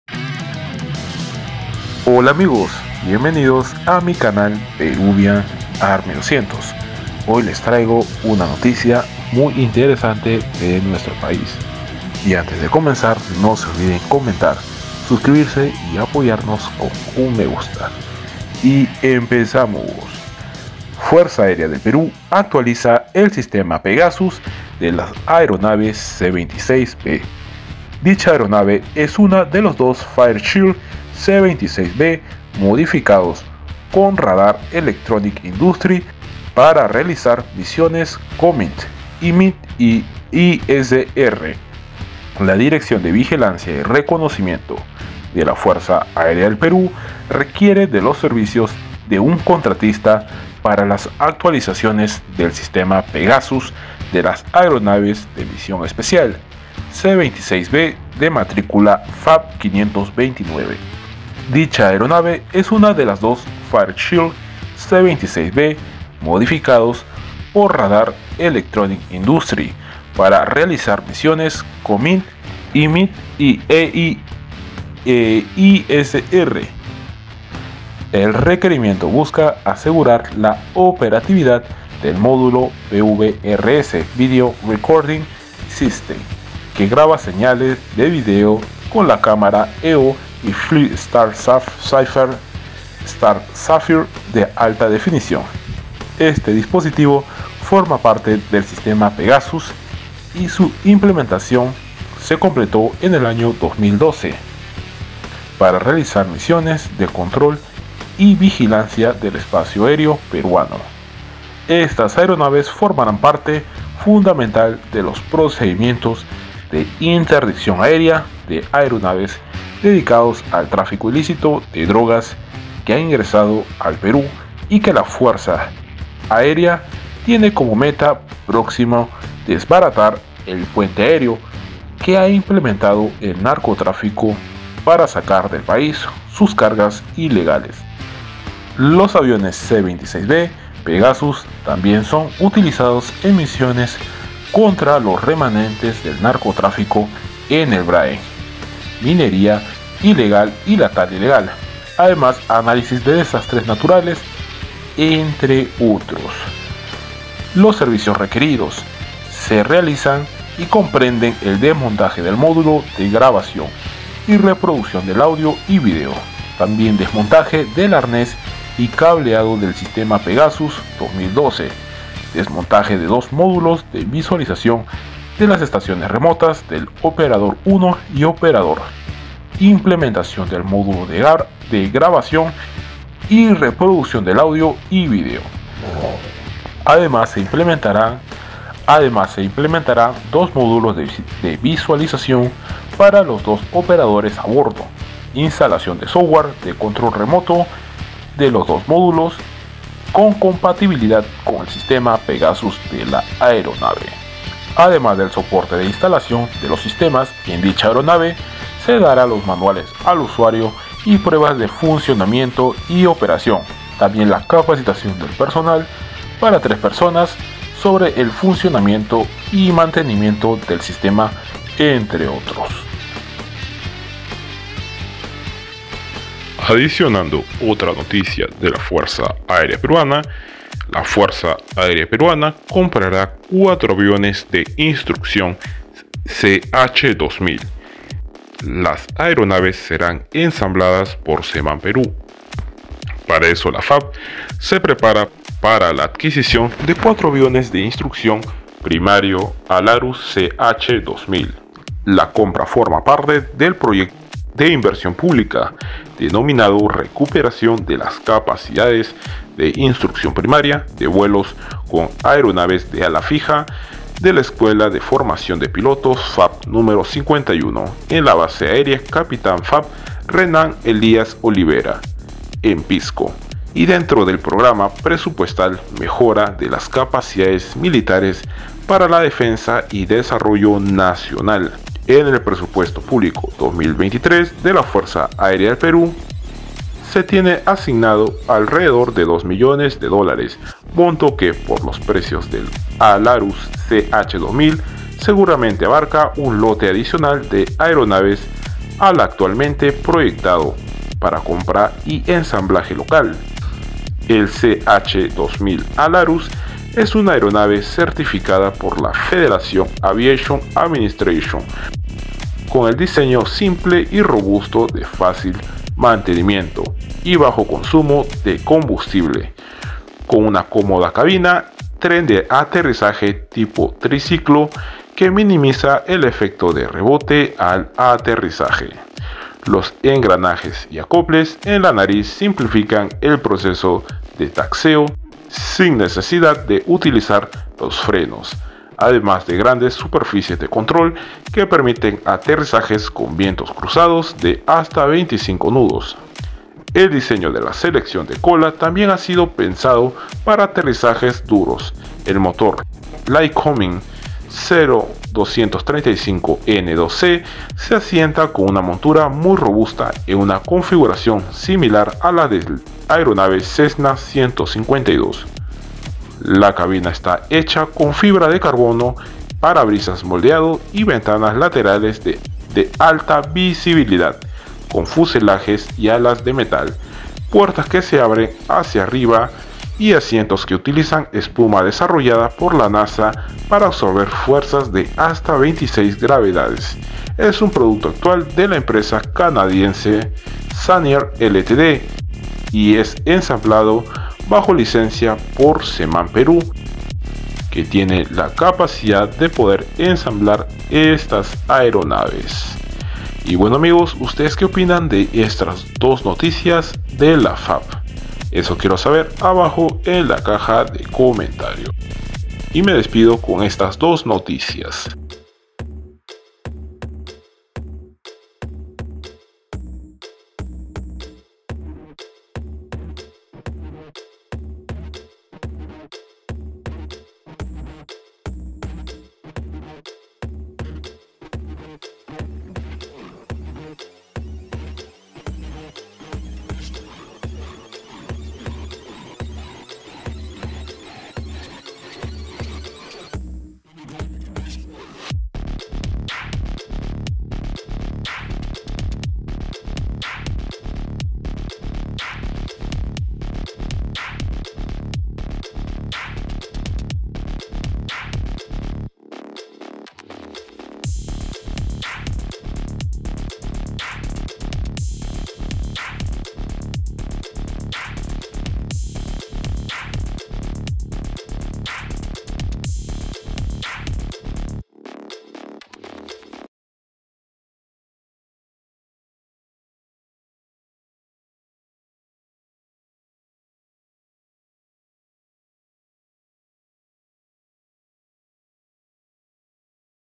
NOTICIAS FAP Compra 4 Ch-2000 Y Modernización Fairchild C-26B ‼️😱🇵🇪
hoy les traigo dos noticias relacionados con la Fuerza Aérea  del Perú.